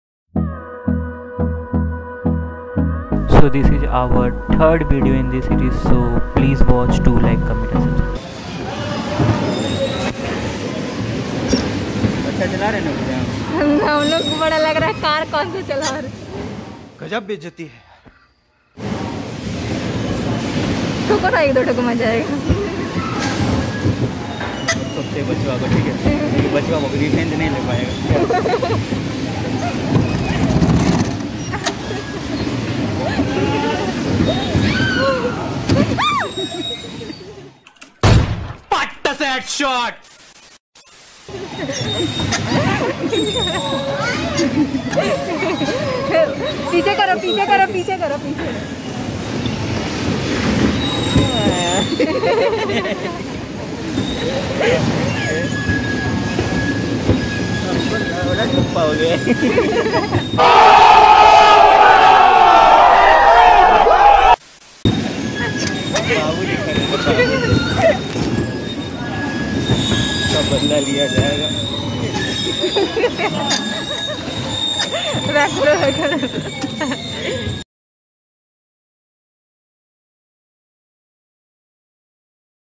Striking car at Nicco Park | Car fight nicco park | Nicco Park All rides | Best Amusement Park
Striking car at Nicco Park , Nicco Park Kolkata | Scary Roller Coaster | Best Amusement Park In India | Kolkata

In this video we are taking the striking car ride in nicco park I had covered all the rides in nicco park in video formats so plz do watch before visit so that you will never miss any ride.